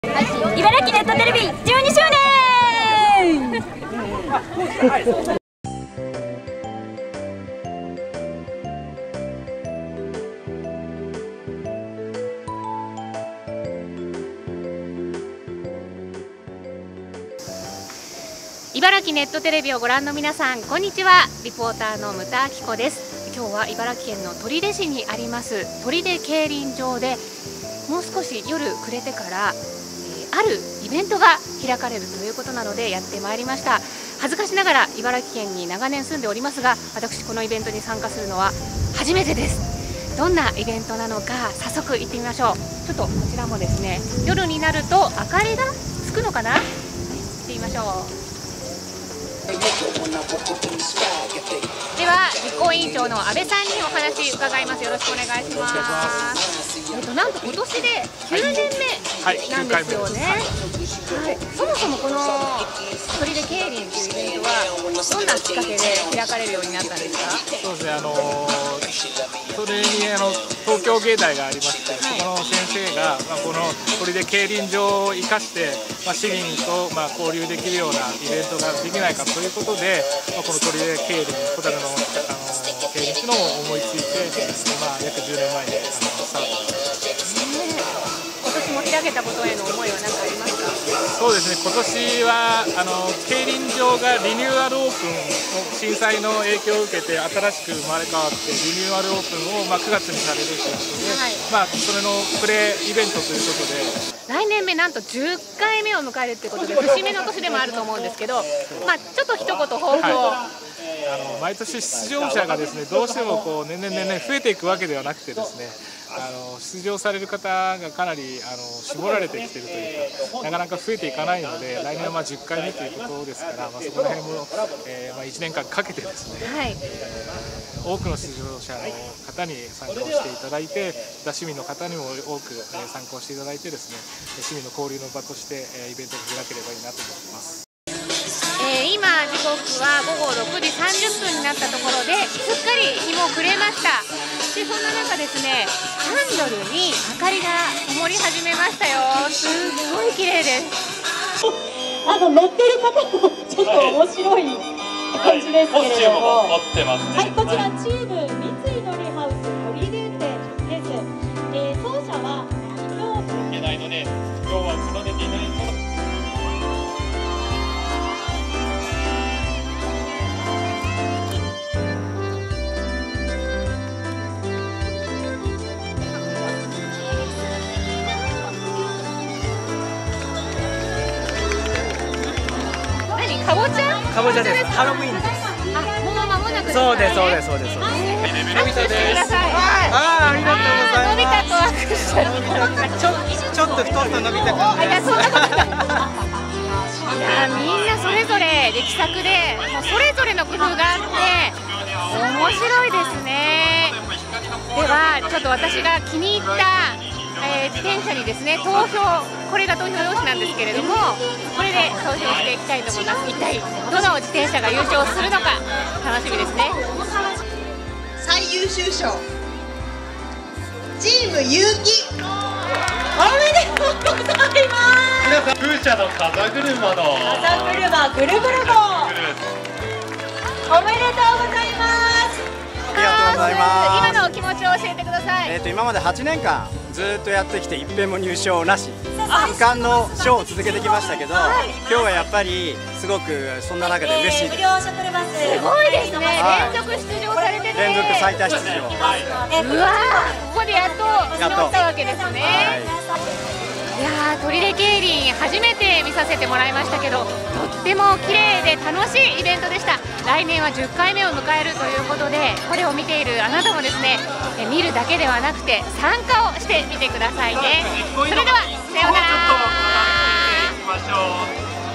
茨城なるべく速報「第9回取手蛍輪開催！（2013年8月31日）
今回の茨城なるべく速報は、今年で第9回目となる「取手蛍輪」をレポートします！それぞれ趣向を凝らした「電飾」自転車でその美しさ、楽しさを競います！ぜひ来年の10回大会にはアナタも参加してみませんか？